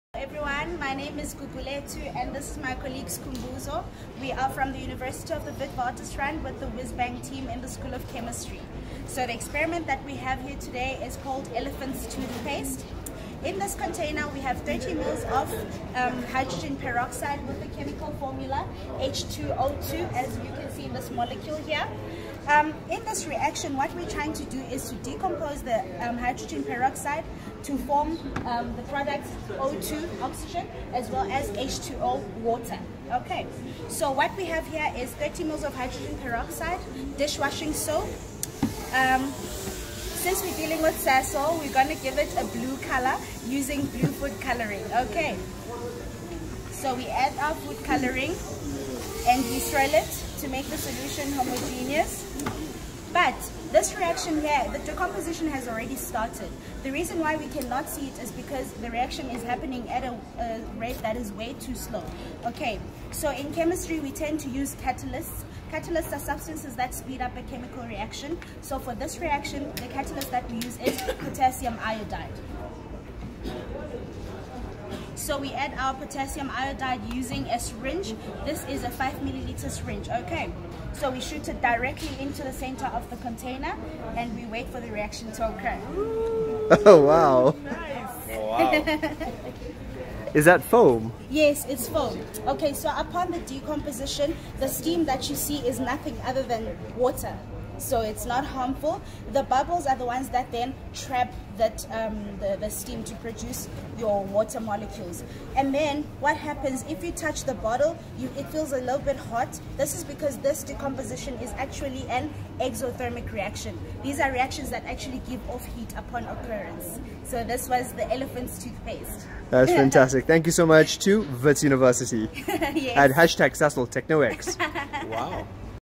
How to make elephant's toothpaste? SasolTechnoX 2018
Wits University WhizzBang Team member Gugu Charmaine demonstrates how to make elephant's toothpaste for inquisitive learners at SasolTechnoX exhibition.

In this experiment, the aim is to carry out a reaction of decomposition of Hydrogen Peroxide (H2O2) into Oxygen (O2) and water (H20). The starting materials are 30mL if peroxide (50% concentration) and 5mL of our household dishwashing liquid. The decomposition reaction happens with these two substances but it occurs so slowly that it cannot be observed by the naked eye. In order to make the reaction happen faster, we employ the use of a catalyst, in this case it’s Potassium Iodide (KI). Therefore we add 5mL of KI into our peroxide-soap mixture, giving us the big foam that we observe. The bubbles act as a trap for the oxygen gas and the water product is given off as steam. The reaction is an exothermic one, meaning that it gives off heat as it proceeds. This heat is sufficient to turn the water into steam.